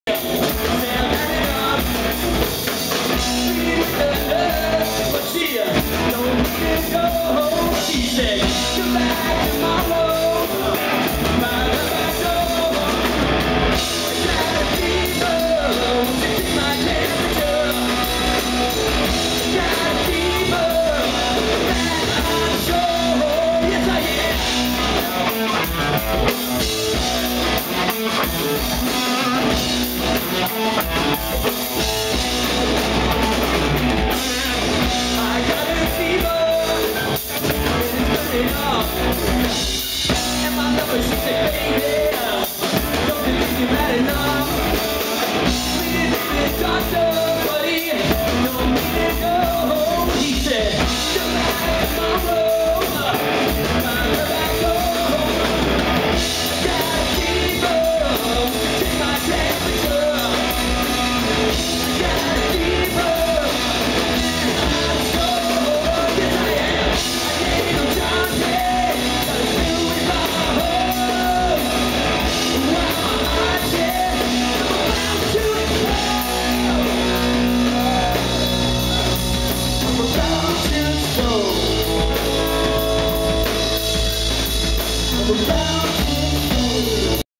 NXNE June 21 2009 C'Mon
NXNE Festival Toronto June 21 2009 C'Mon at Lee's Palace